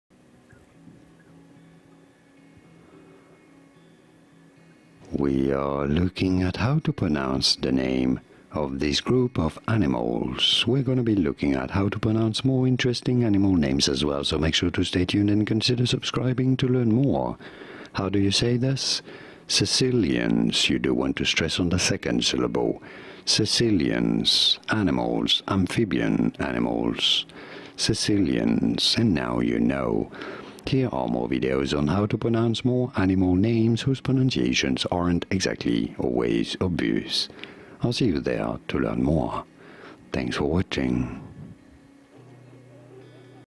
How to Pronounce Caecilians? (CORRECTLY)
This video shows you how to pronounce Caecilians (pronunciation guide).

Caecilians Amphibians
Caecilians are a group of limbless, vermiform or serpentine amphibians. They mostly live hidden in the ground and in stream substrates, making them the least familiar order of amphibians. Wikipedia Source